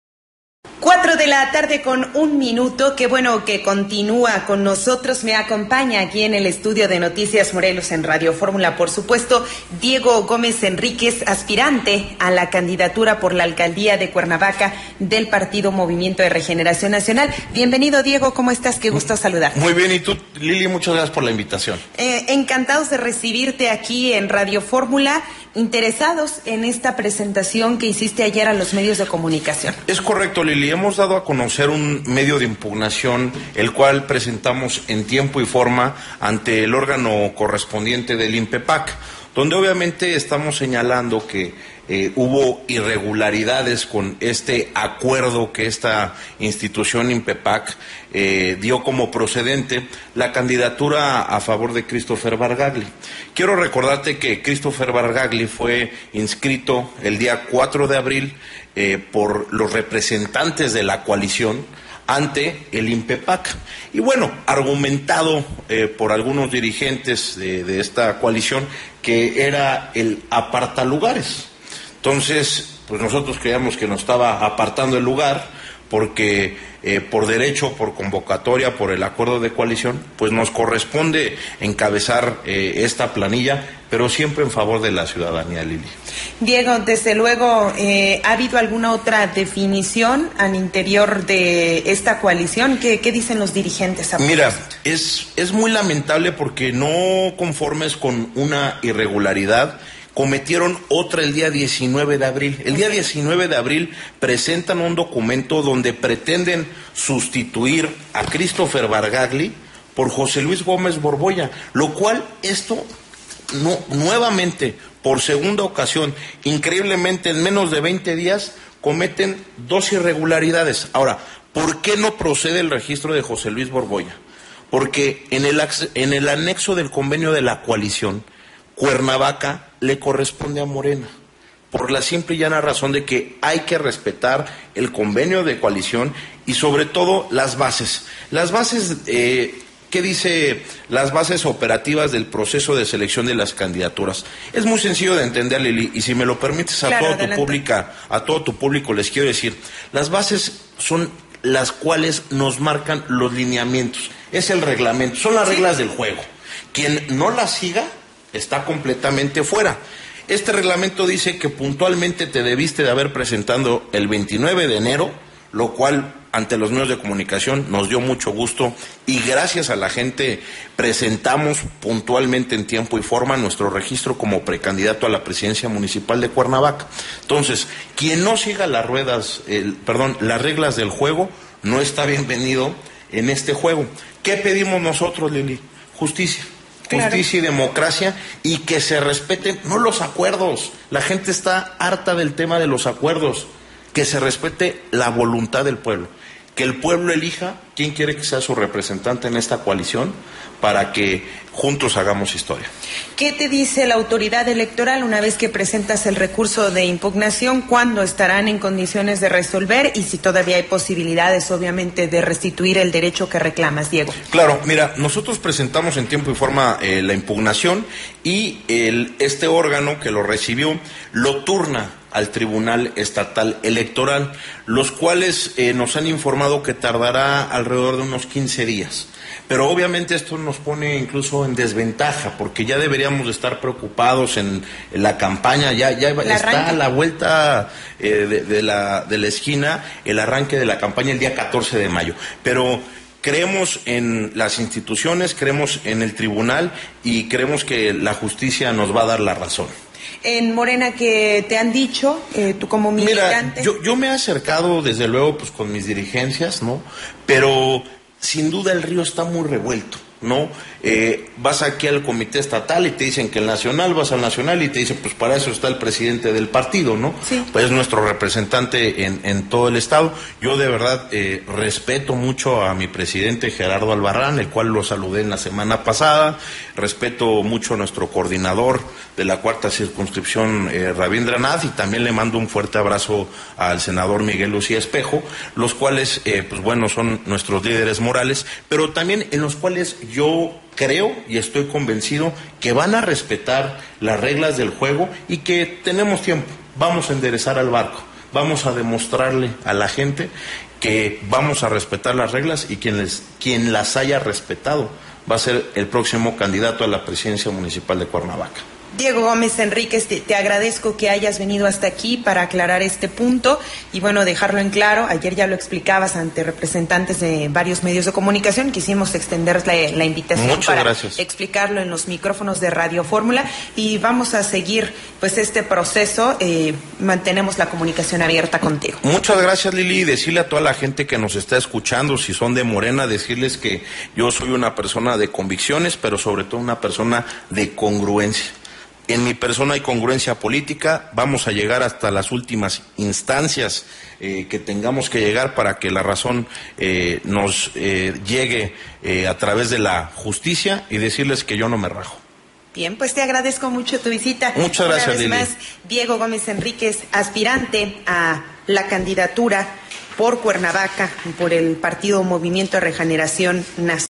27/abr/2018 En Entrevista Diego Gómez Henríquez
Precandidato a la presidencia municipal de Cuernavaca por MORENA